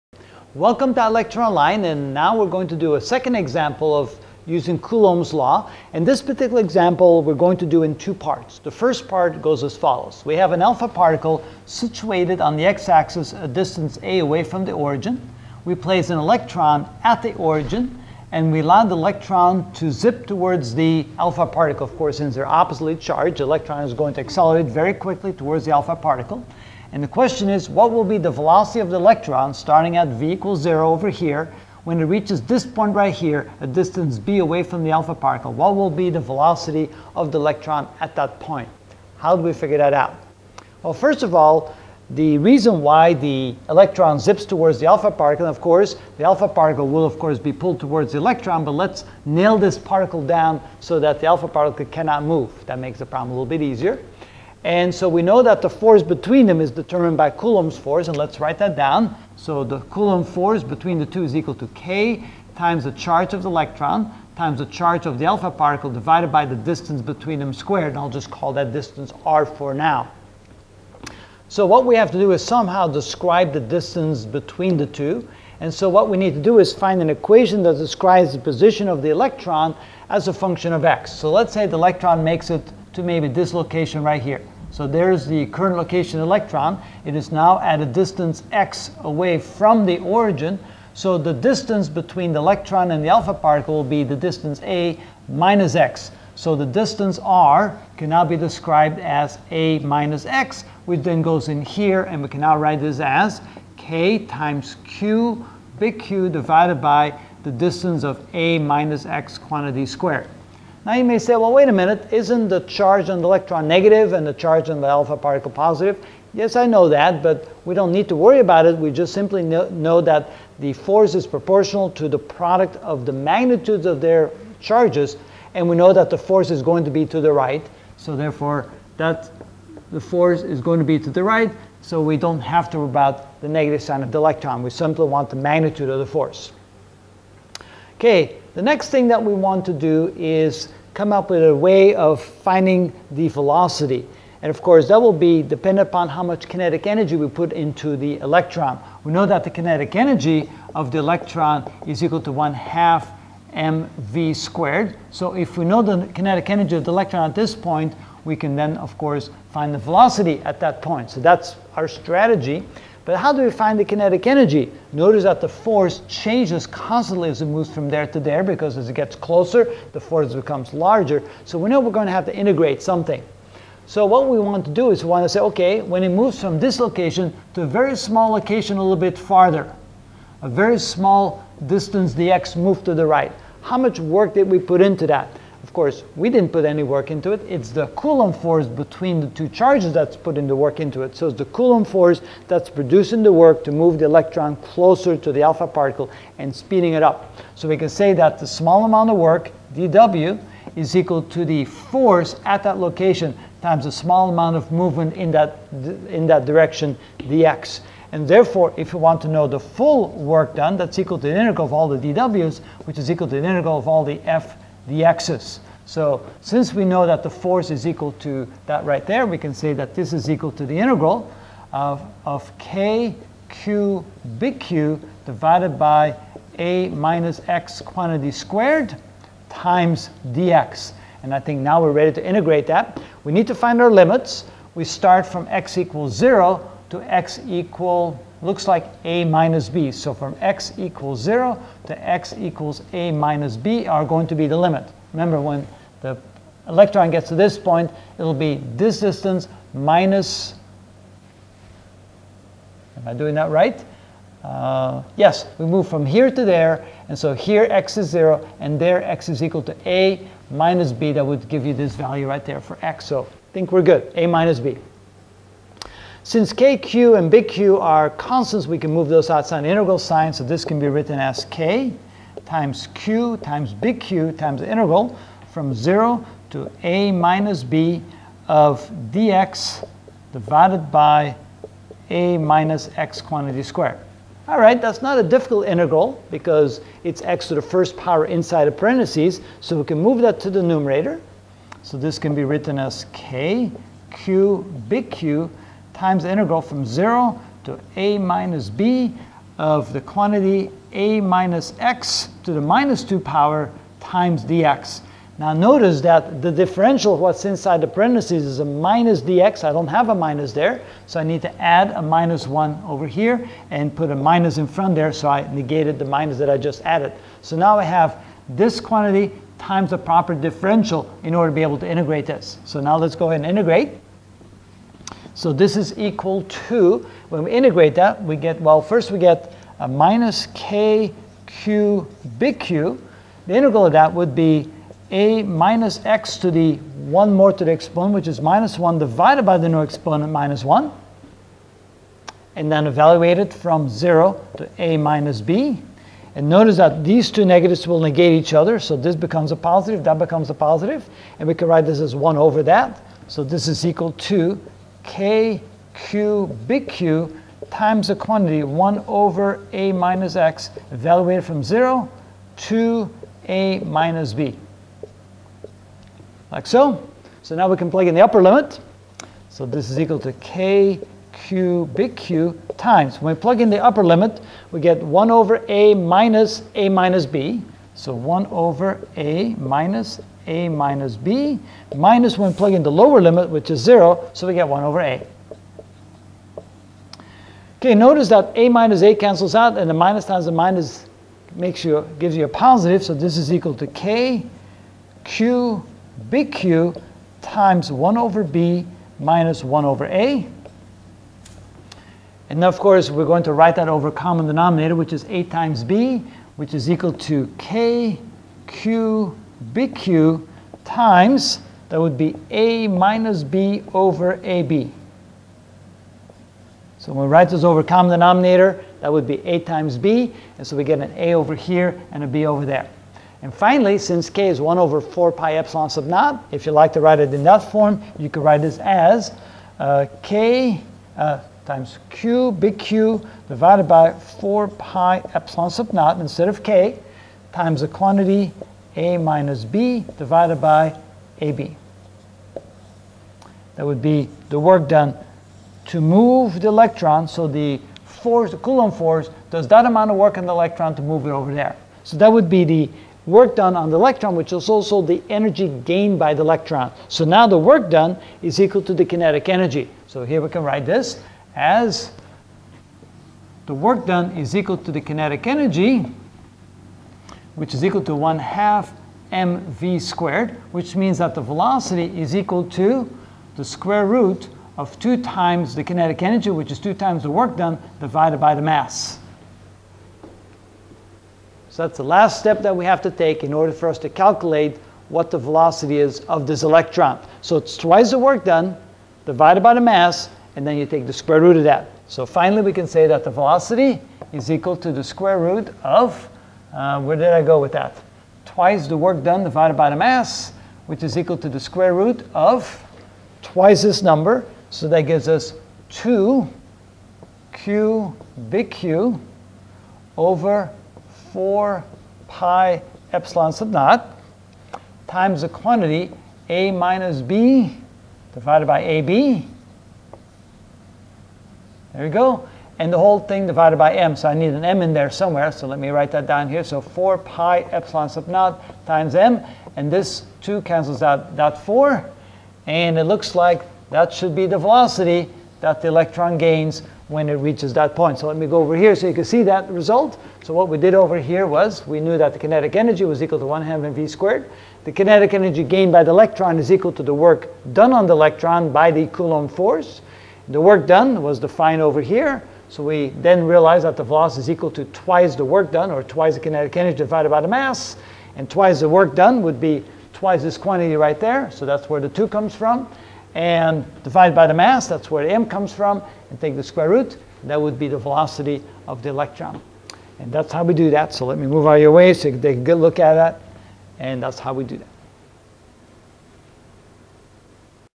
Physics 35 Coulomb's Law (5 of 8) Example 2A (Challenging Problems)
In this video I will find velocity(electron) = ? at a distance "b" from the alpha-particle.